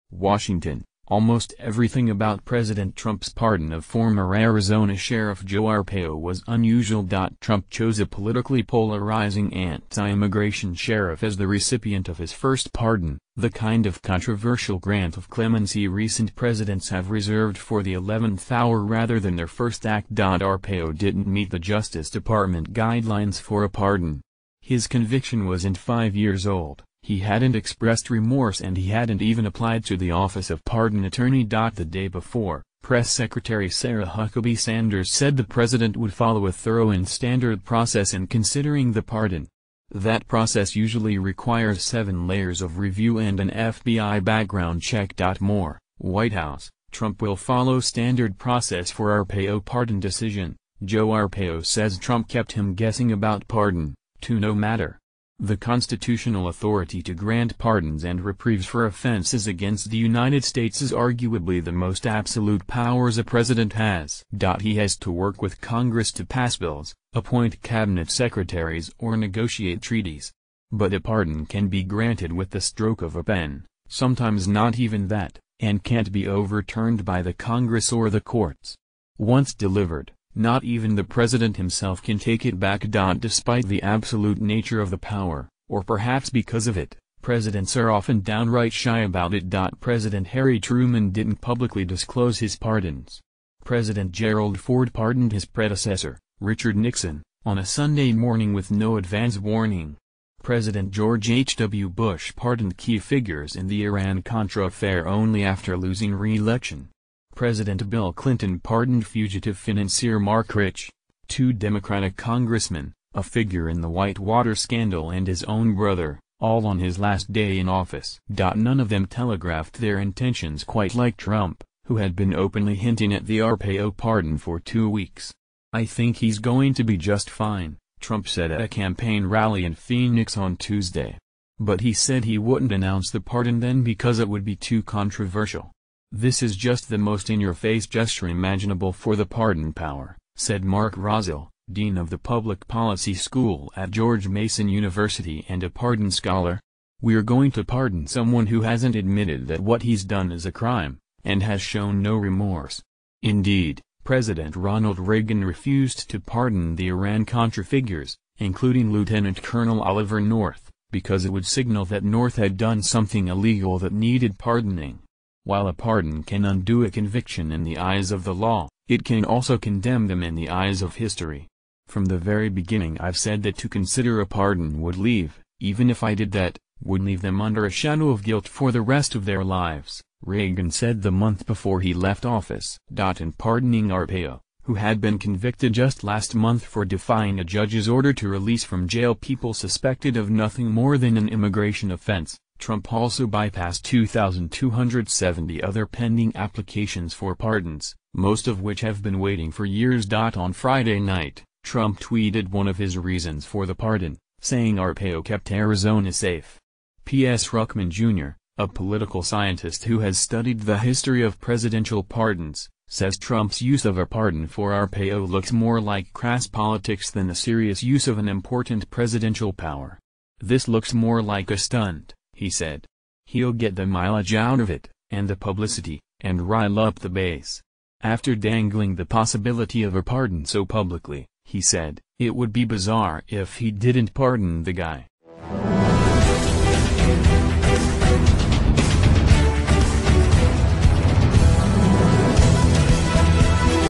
Analysis: Trump's bold Joe Arpaio pardon breaks with presidential tradition
Welcome to Breaking News : News Trending TV